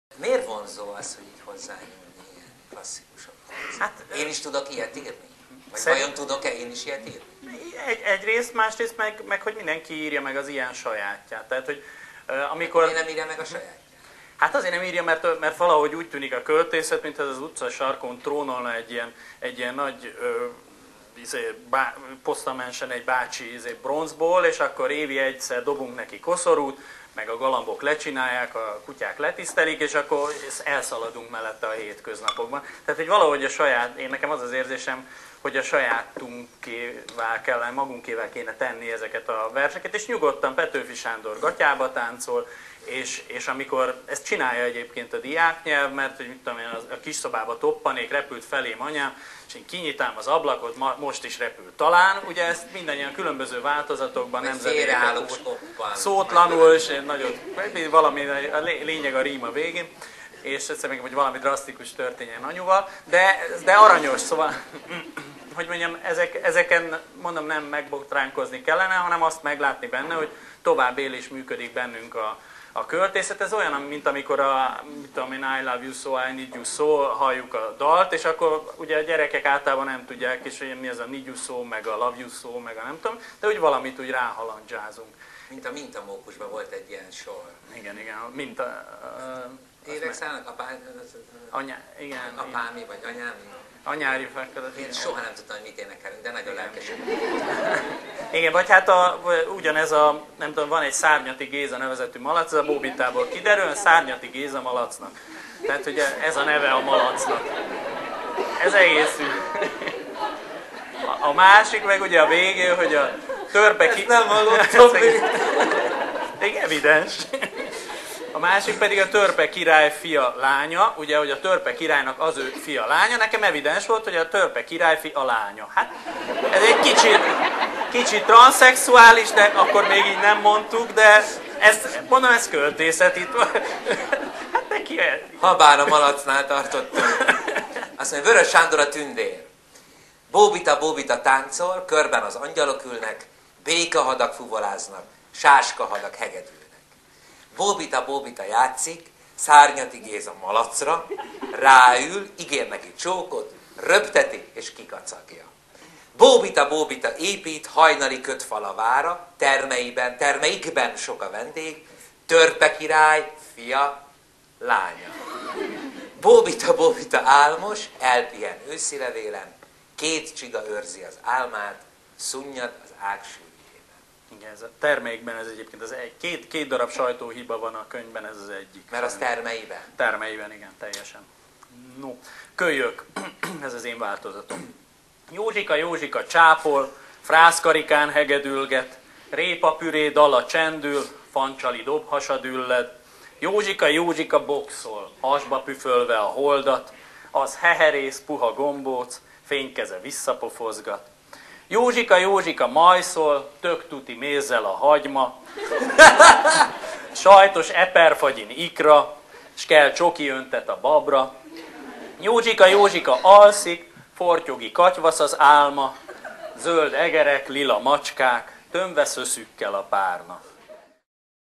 A lényeg a rím a végén
A Képzett társítások - Aegon-estek legutóbbi vendége Lackfi János a gyerekköltészet örökzöldségéről és örök zöldségeiről beszélt, majd a végén felolvasta a maga átköltését is. (2010)